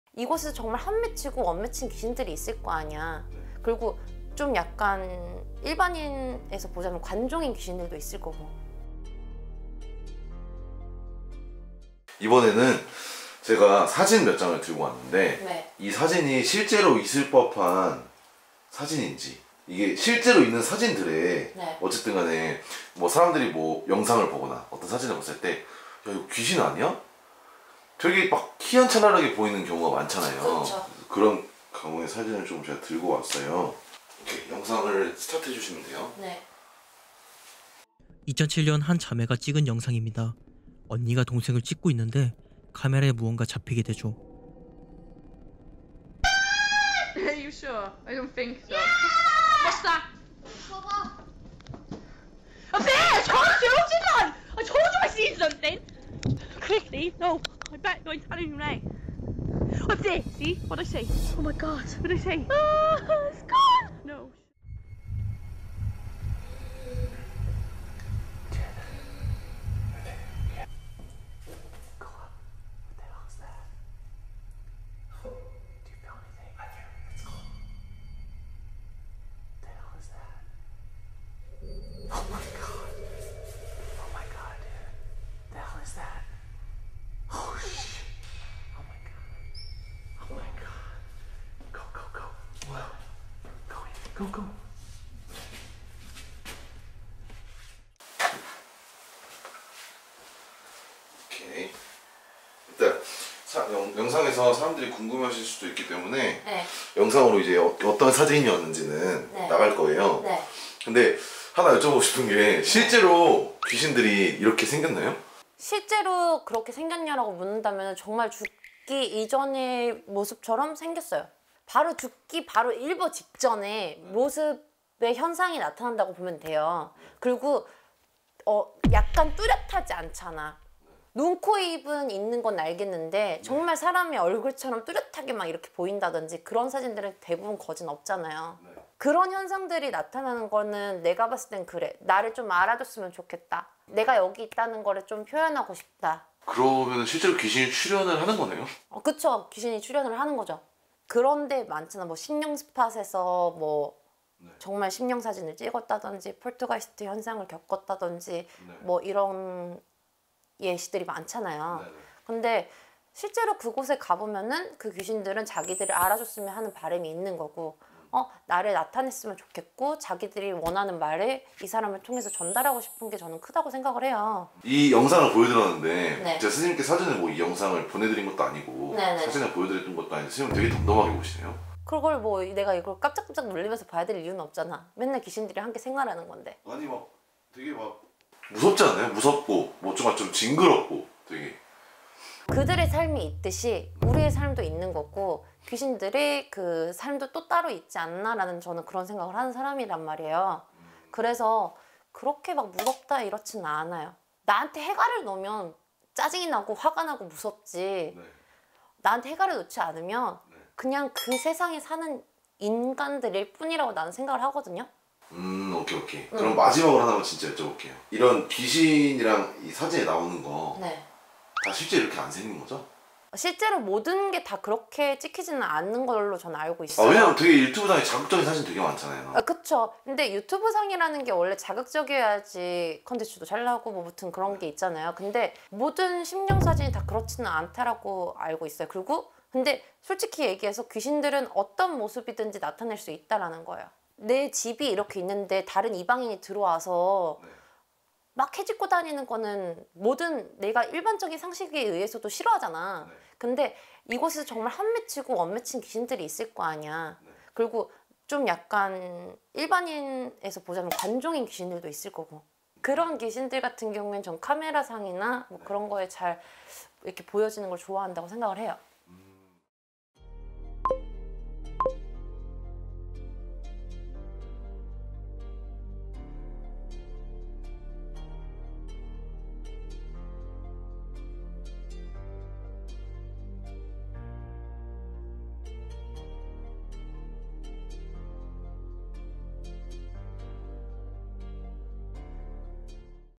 수많은 귀신영상 귀신사진들 구분하는 방법 알려드립니다! "귀신들도 관종이다ㅋㅋ?" [남양주점집 수련애기씨]
자막

★대표블로그★
-올마이티TV